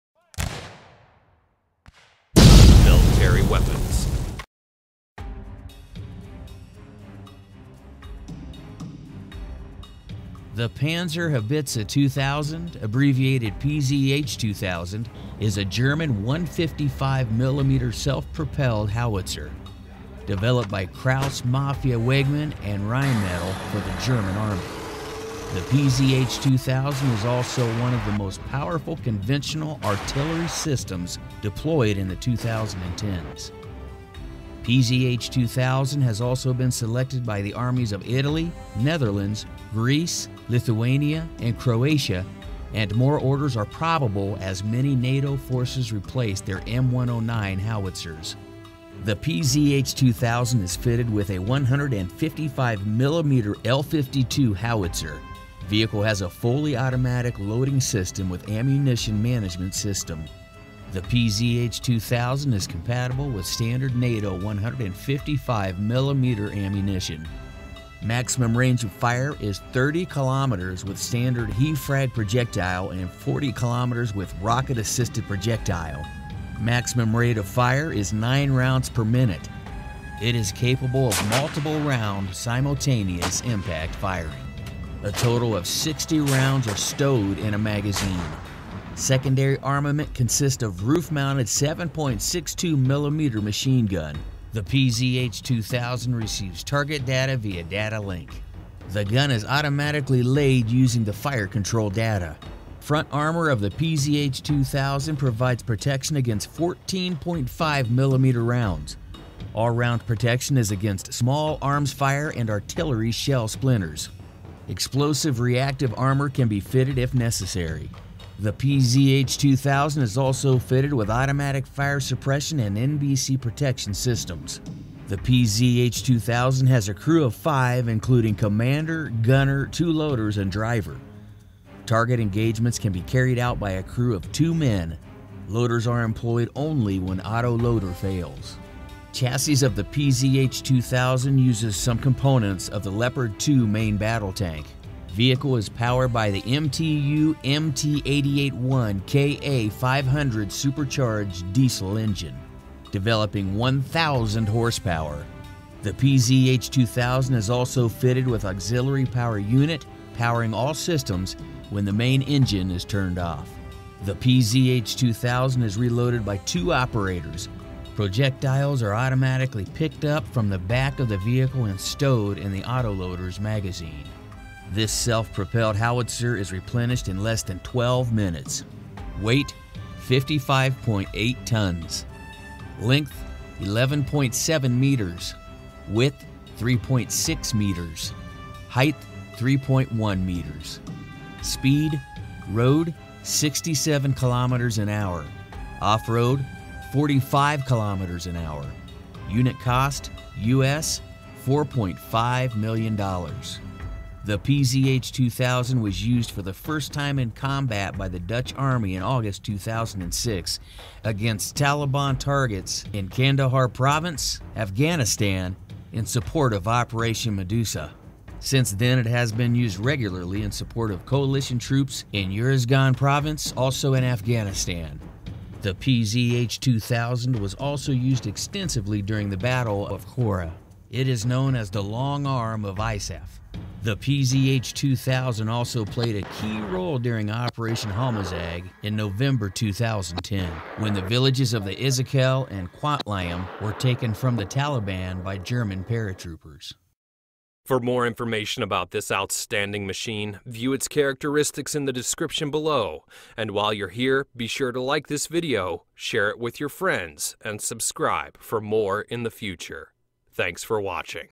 Panzerhaubitze 2000 (PZH 2000): German 155mm Self-Propelled Howitzer
The PzH 2000 is one of the most powerful conventional artillery systems deployed in the 2010s.
The Panzerhaubitze 2000 ("armoured howitzer 2000"), abbreviated PzH2000, is a German 155mm self-propelled Howitzer developed by Krauss-Maffei Wegmann and Rheinmetall for the German Army. It is particularly notable for a very high rate of fire; in burst mode it can fire three rounds in 9 seconds, ten rounds in 56 seconds, and can—depending on barrel heating—fire between 10 and 13 rounds per minute continuously. The PzH 2000 has automatic support for up to 5 rounds of Multiple Rounds Simultaneous Impact (MRSI). The replenishment of shells is automated. Two operators can load 60 shells and propelling charges in less than 12 minutes. PzH 2000 has also been selected by the armies of Italy, Netherlands, Greece, Lithuania and Croatia, and more orders are probable as many NATO forces replace their M109 howitzers.

Type: Self-propelled artillery
Place of origin: Germany

Designed: 1996
Unit cost: US$4.5 million
Produced: 1998-present

SPECIFICATIONS
Weight: Combat: 55.8 t (61.5 short tons)
Length: 11.7 m (38 ft 5 in)
Width: 3.6 m (11 ft 10 in)
Height: 3.1 m (10 ft 2 in)
Crew: 5 (commander, driver, gunner, and two loaders)

Armor:
welded steel, 14.5 mm resistant
additional bomblet protection

Main Armament:
Rheinmetall 155mm L52 Artillery Gun
(60 rounds)
3 rounds in 9.0 seconds (Burst)
10 round/min

Secondary Armament:
Rheinmetall MG3 7.62 mm machine gun

Engine:
MTU 881 Ka-500
1,000 PS (986 hp, 736 kW)

Power/weight: 17.92 PS/t
Suspension: torsion bar
Operational Range: 420 km (261 mi)

Speed:
Road: 67 km/h (41 mph)
Off-road: 45 km/h (28 mph)

Artillery Systems & Ballistic Missiles || Military Weapons